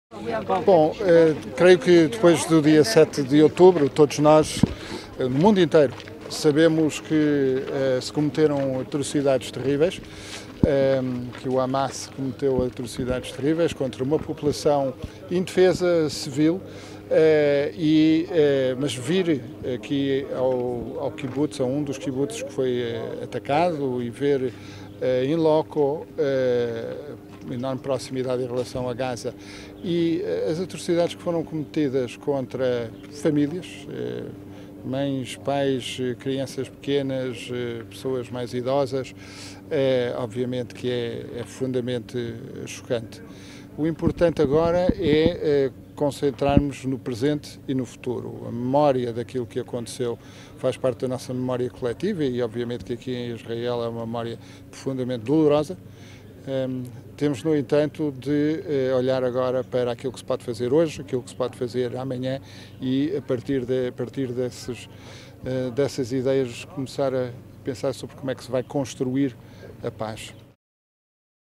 "O tempo congelou" num 'kibbutz' atacado perto de Gaza
Casas incendiadas e ruas cobertas de destroços, mobílias, roupas e brinquedos marcam hoje o cenário do ‘kibbutz’ Kfar Aza, sul de Israel, atacado pelo Hamas em 07 de outubro, e que era antes “95% Céu e 05% Inferno”.